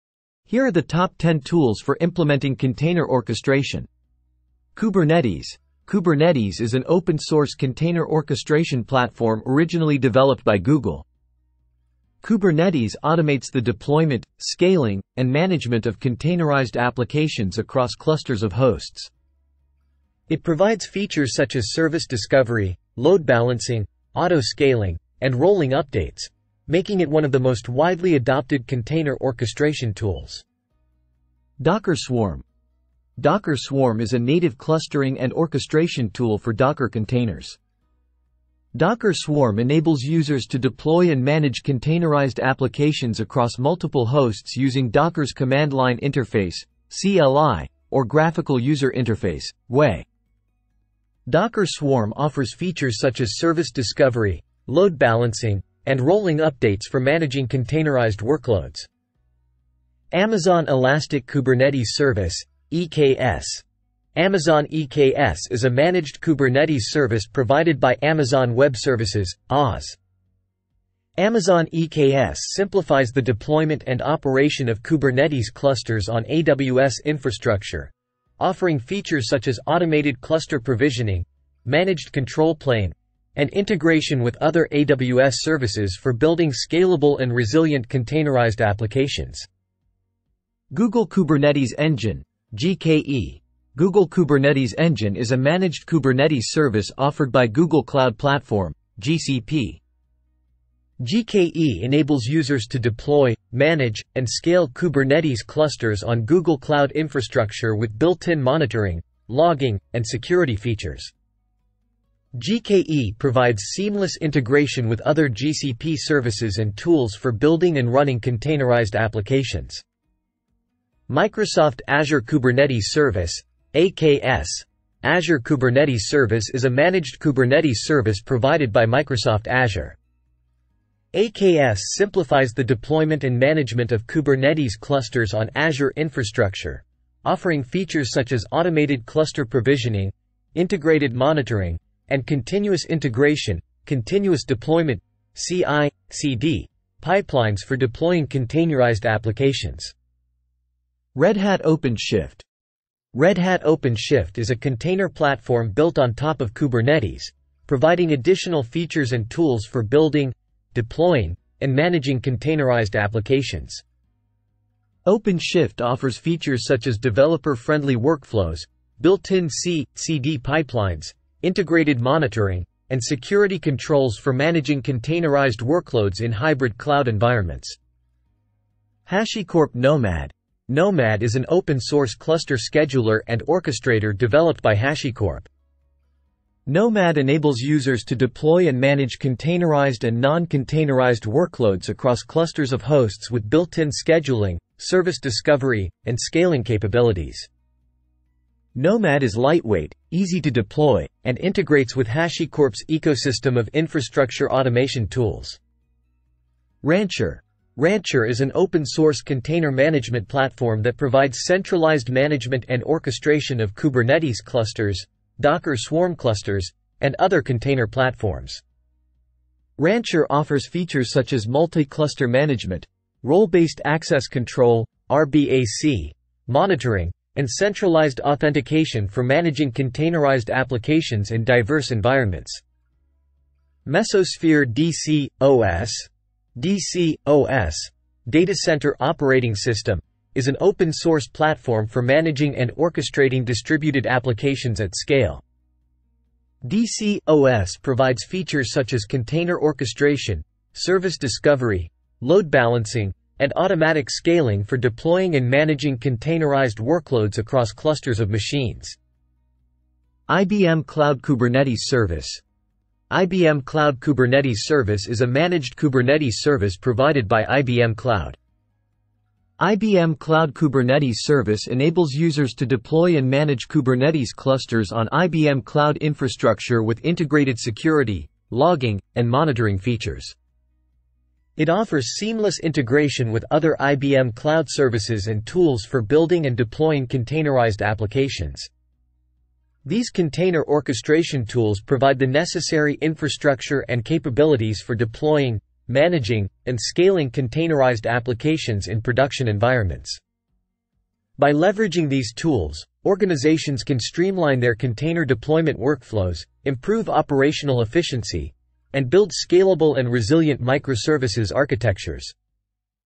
Top 10 Tools for Implementing Container Orchestration
Explore essential tools for implementing container orchestration, enabling efficient management, deployment, and scaling of containerized applications in cloud-native environments.